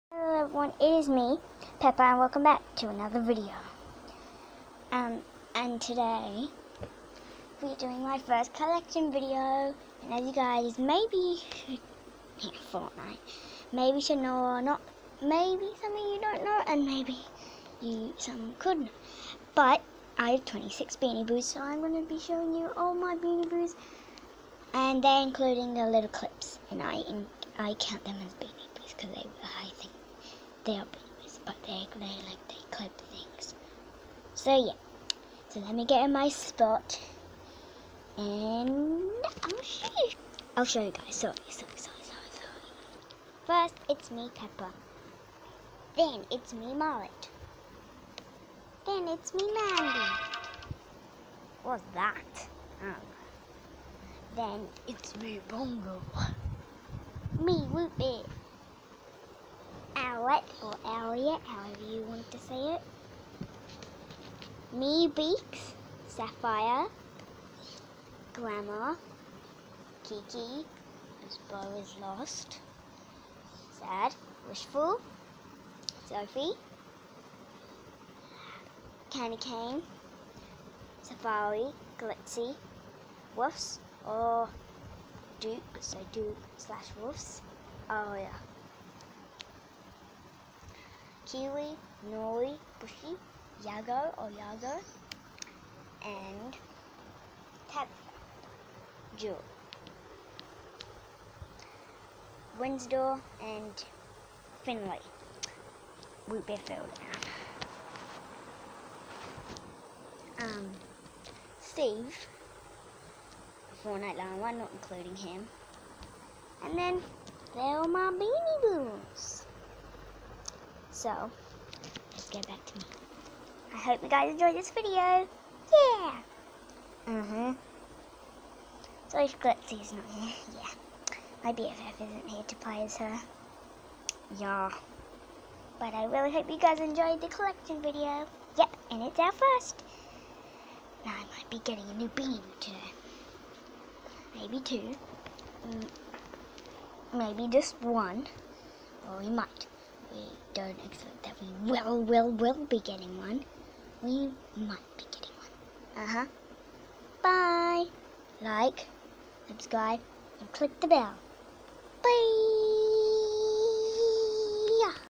My beanie boo collection 26 boos!
My collection is here!! I know I haven't got like 50 as that is my starter goal for now but I think it's good enough for now 🙂 and who's your favorite BB out of my collection?